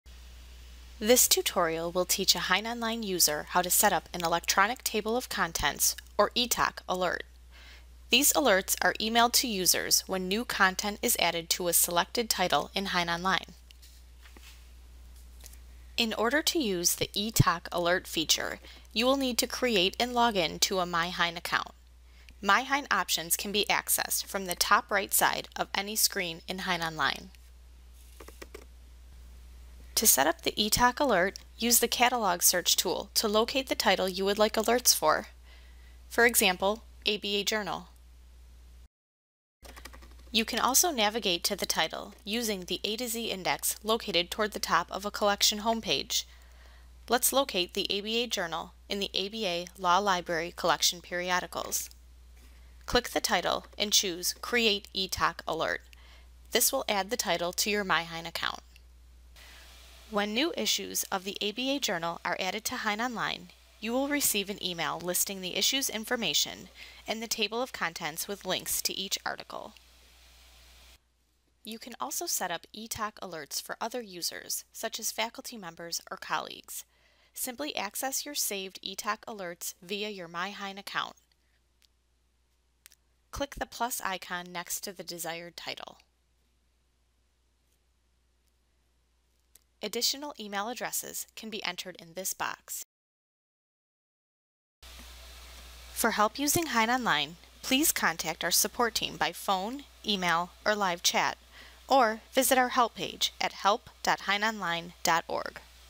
How to Set up e-TOC Alerts in HeinOnline
Brief tutorial explaining how to set up electronic table of contents alerts in HeinOnline.